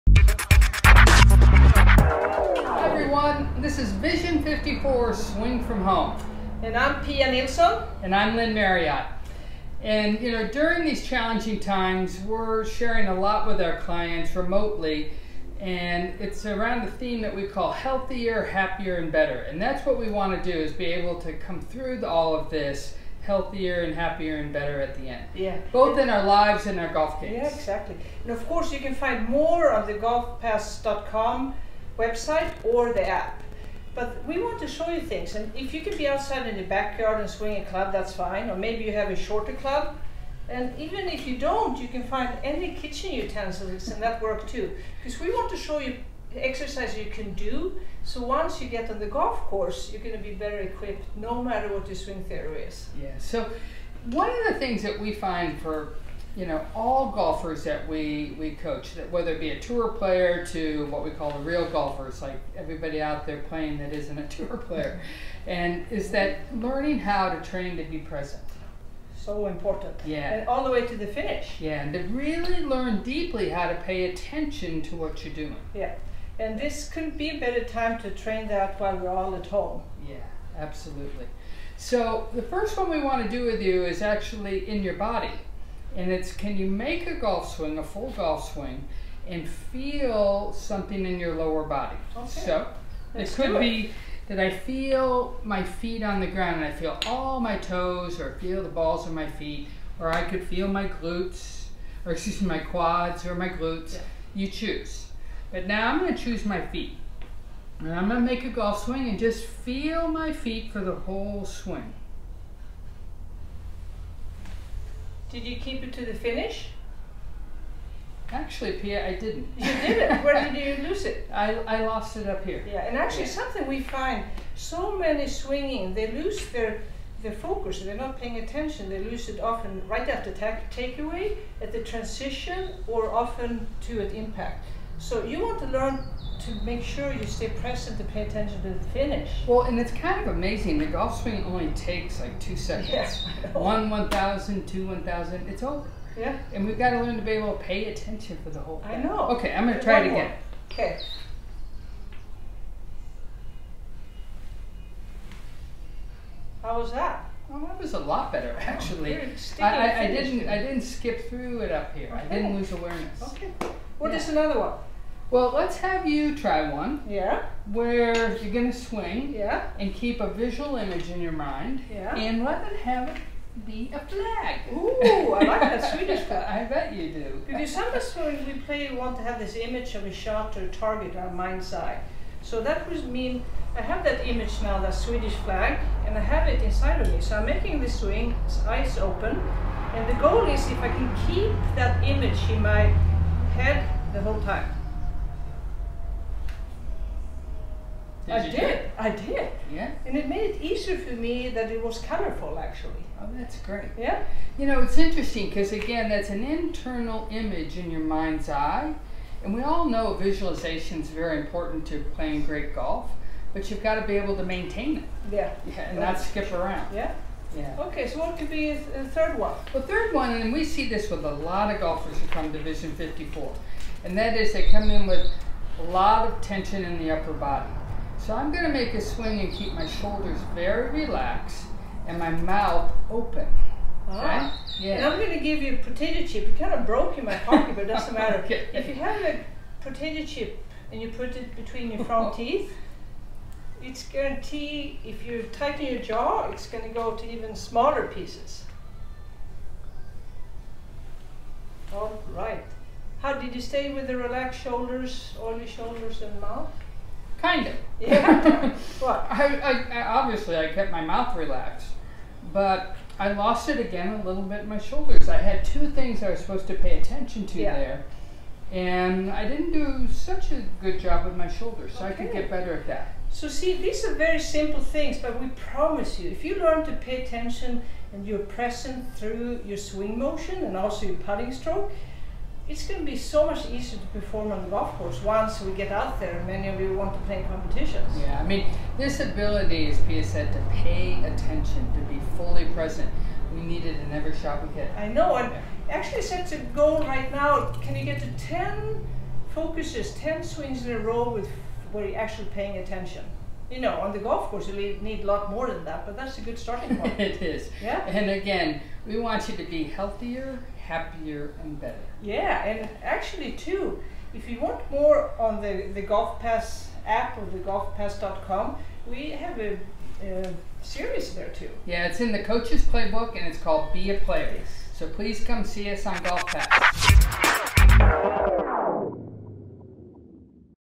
VISION54 - Stay Home - train your Play Box
Train your focus, your ability to pay attention. You can do it in side, outside, anywhere.